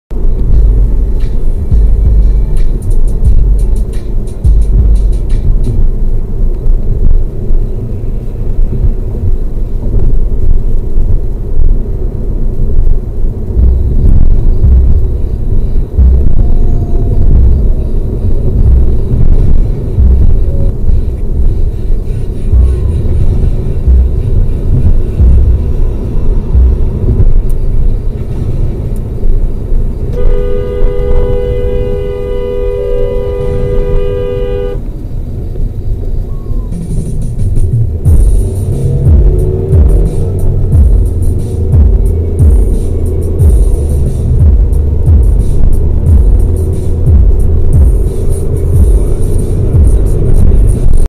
LUBLIN - Wymuszenie przy zmianie pasa ruchu al Witosa /Tysiąclecia
Następny kretyn, brak słów

źródło: Adam Pe